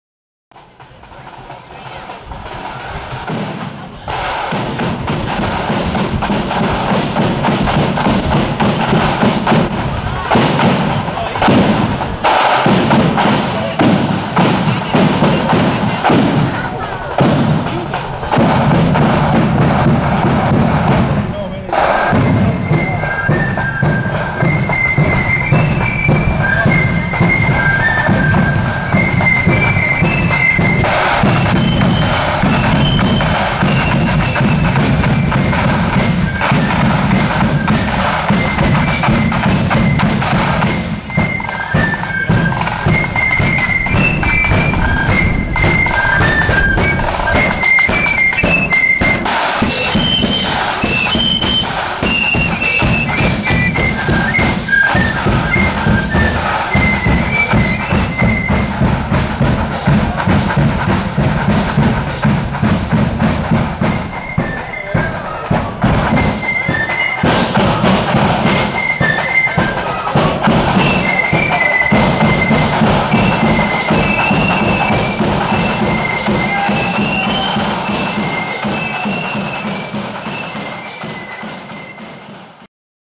East Belfast Protestant Boys 3 @ Ballykeel LSOU Parade 2018
East Belfast Protestant Boys on the march in Ballymena for Ballykeel Loyal Sons of Ulster annual band parade Saturday 19th May 2018